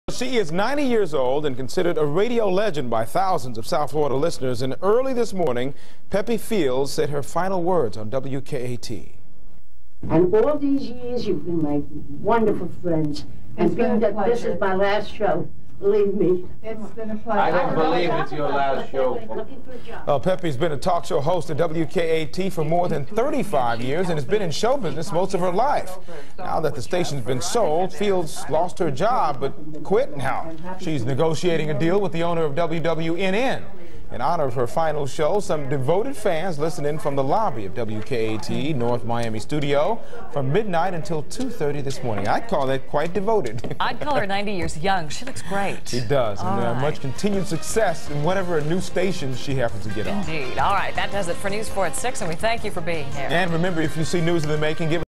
Peppy Fields on WFOR News
A news items about the late Peppy Fields on WFOR News in Miami, FL, when WKAT-AM was sold.